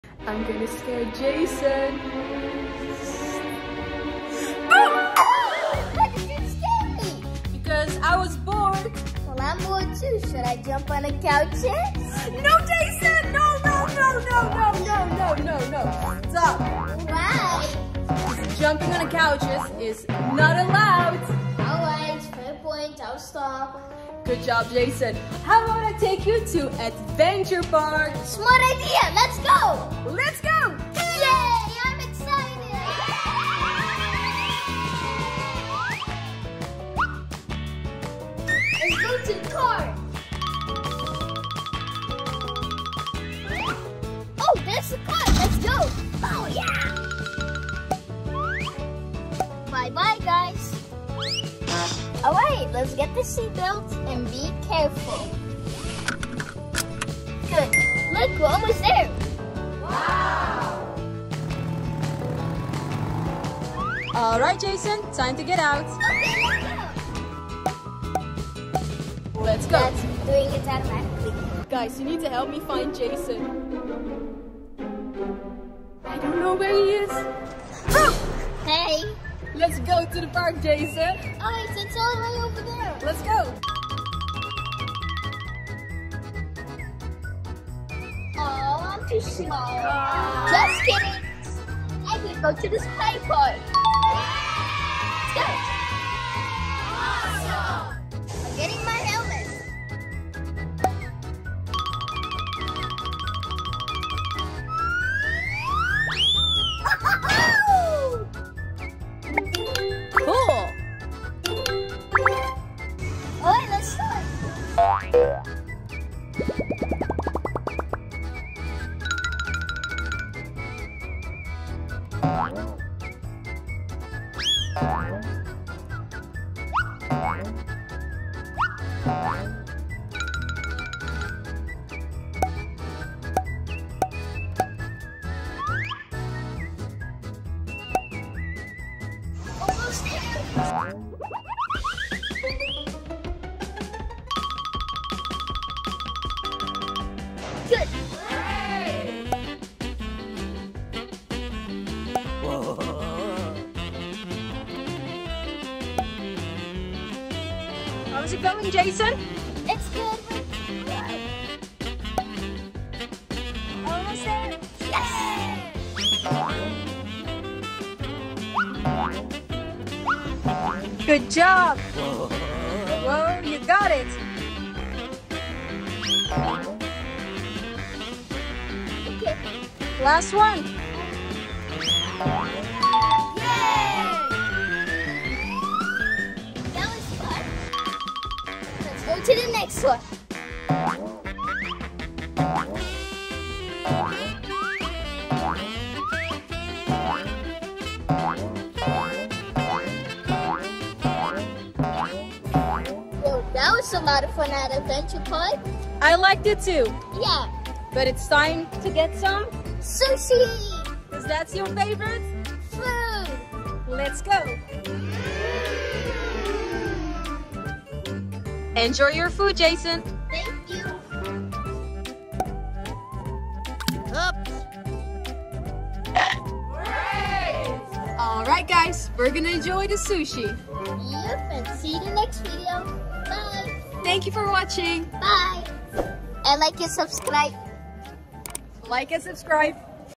Jason play sports in entertainment park vlog
Jason Sara and their new Active entertainment! They both went to play and had fun!